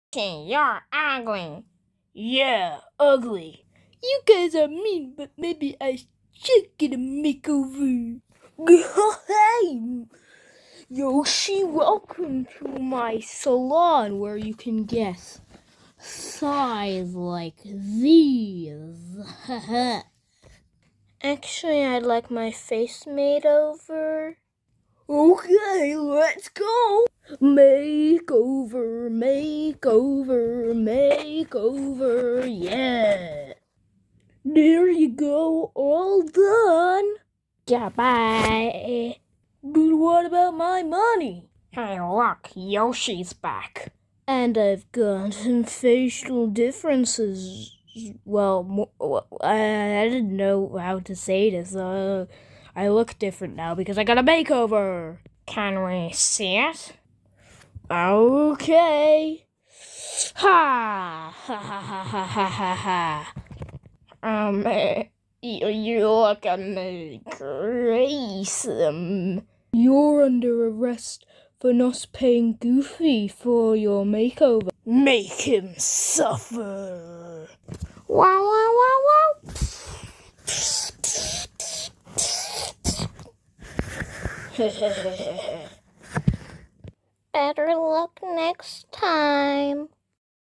yoshi gets a makeover
yoshi decides he needs a makeover to impress Wario and Waluigi